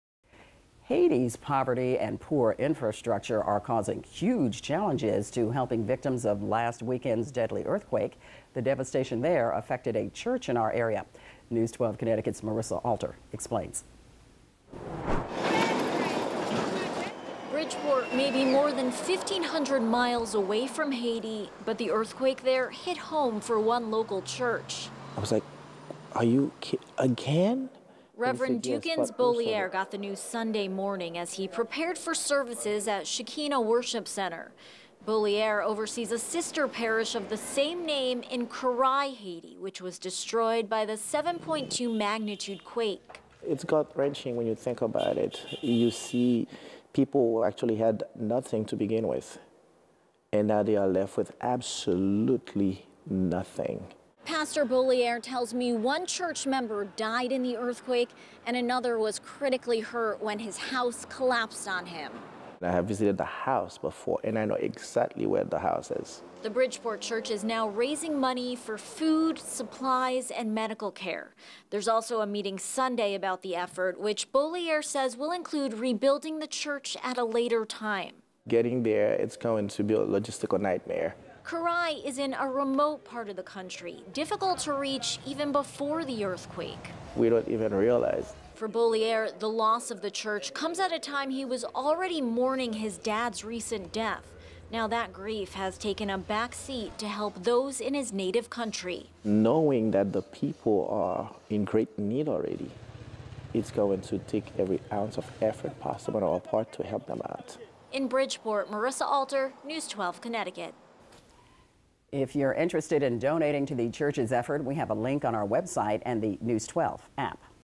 It's gut-wrenching': Haiti earthquake destroys sister parish of Bridgeport church
A Bridgeport church says the recent earthquake in Haiti hit close to home as its sister parish there was destroyed.